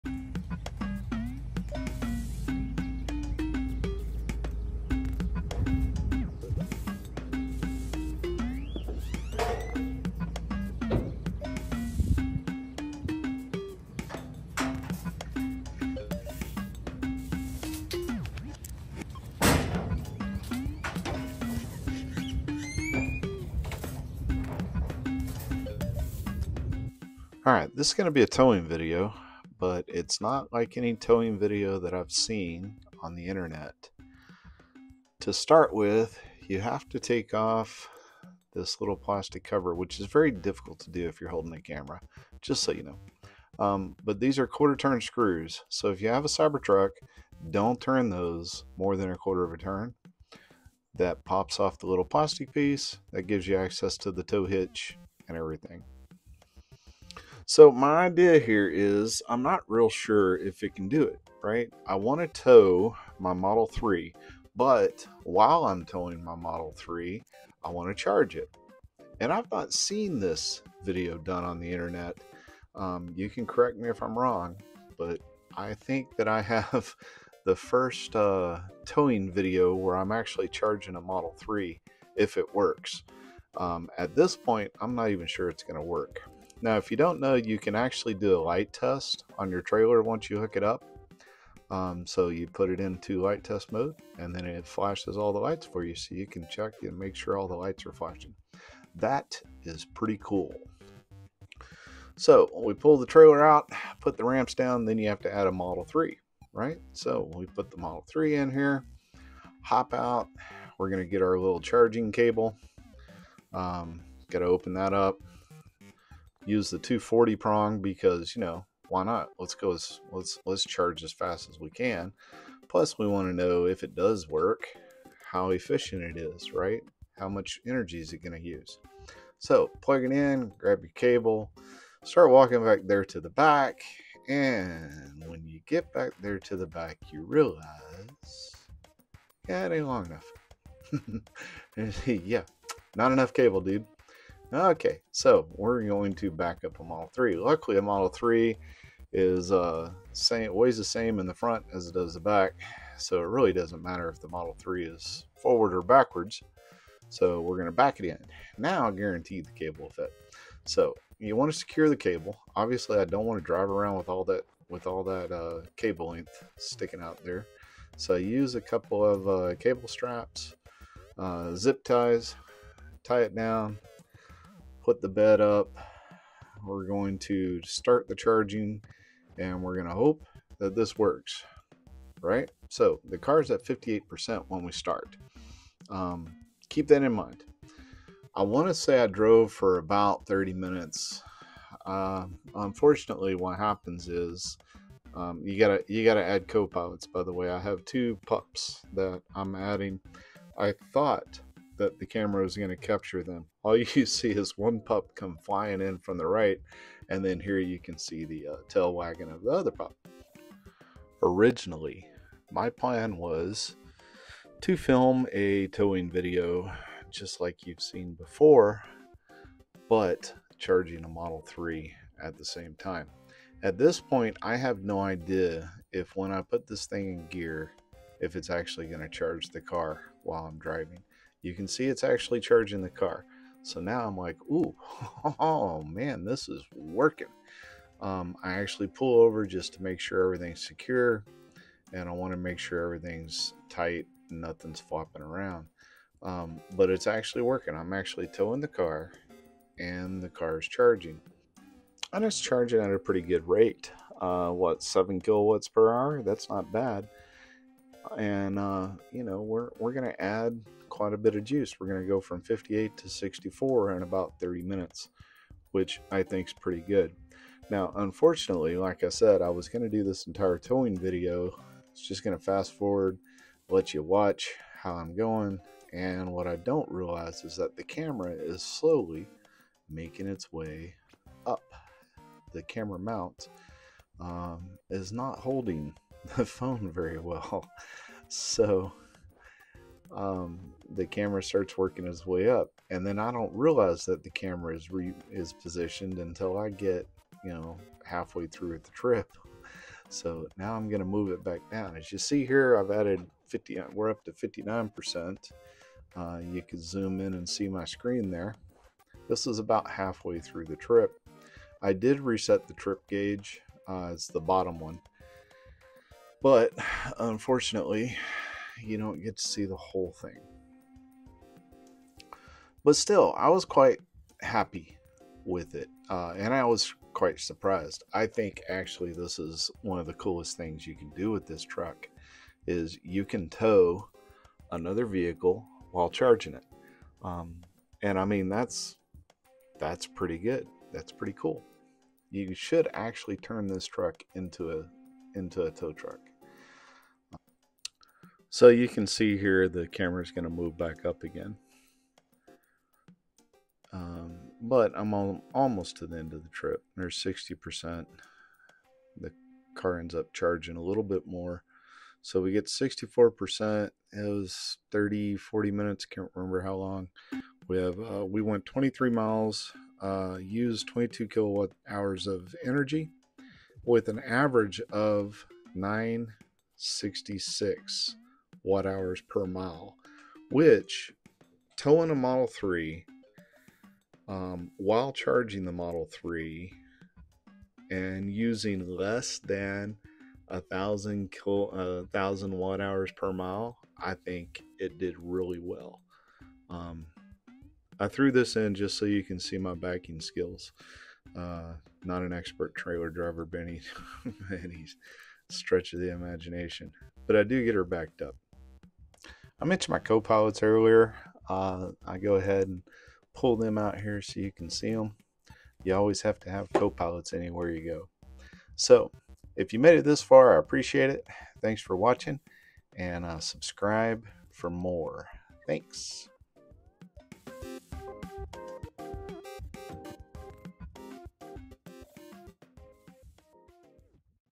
Can a Cybertruck Tow while Charging? (Part 1ish)
Update: OK, I'm going to call this Part 1. My camera moved on me and I didn't figure out a way around it until after I uploaded this video.  Kinda a way around it anyway.   I'm working on Part 2 now.

I know it'll tow a Model 3, but can it tow while charging a Model 3?  Remember, I'm on my own here.  I had camera issues and didn't realize it until I was almost done with the video and I didn't have the trailer long enough to re-record.  So this was what I have for you.  I tow the Model 3 while charging it 75 MPH up a highway for 20 miles.  So, I do more than the 30 MPH the video shows.